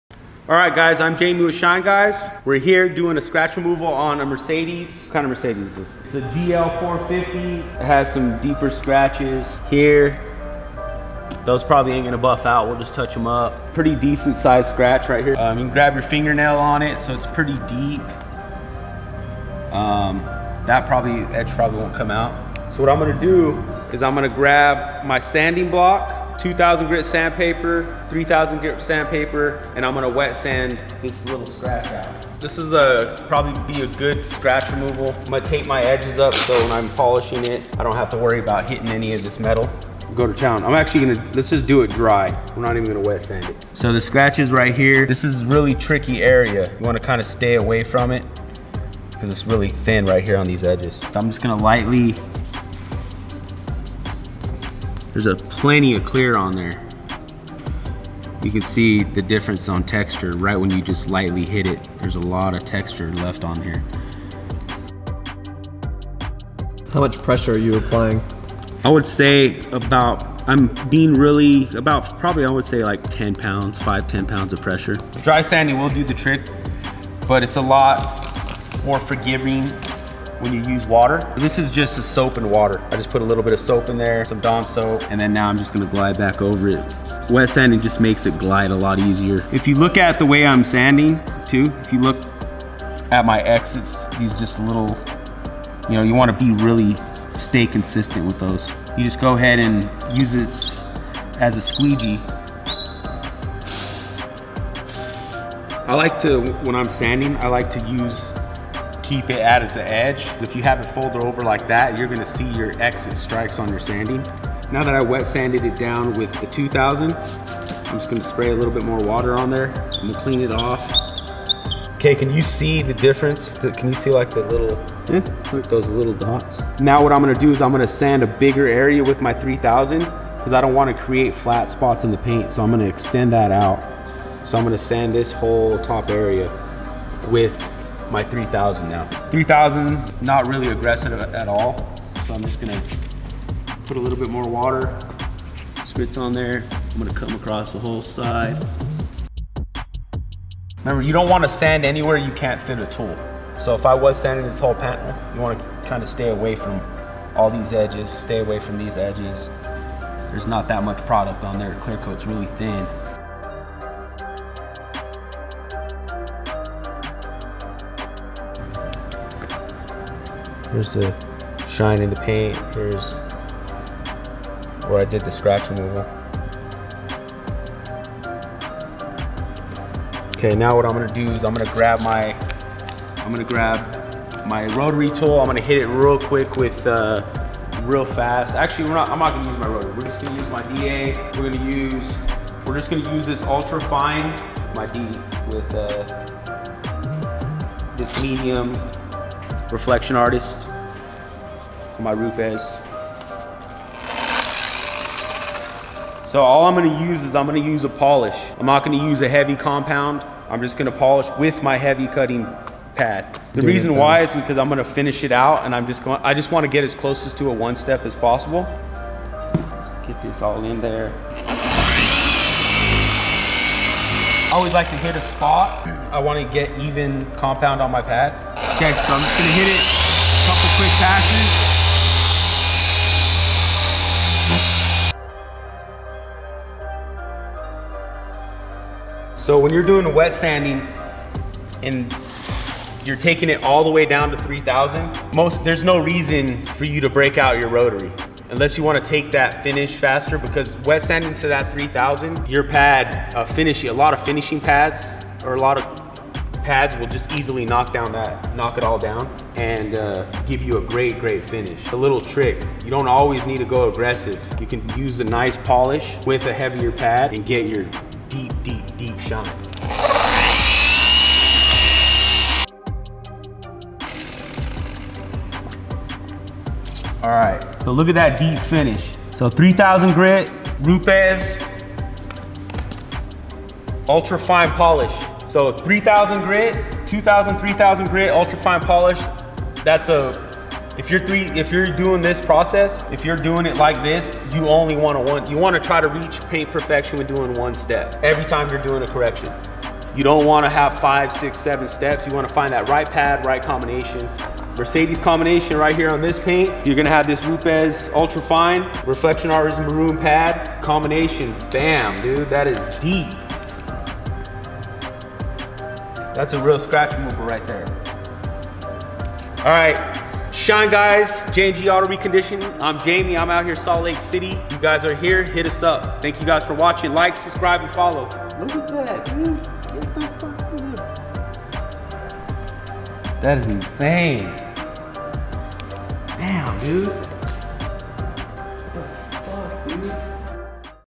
GL450 Mercedes DEEP Scratch Removal! | *BEST TECHNIQUE*
Removing this DEEP SCRATCH on a Mercedes GL450

We cover the BEST technique for wet/dry sanding paint and then buffing out the scratch with a polish and Rupes tool.

Gear we use:
Meguiar Compound M100
Dewalt Rotary Polisher
Rupes Mark III 15mm
Rupes Uno Paint Sealant
Rupes Course Compound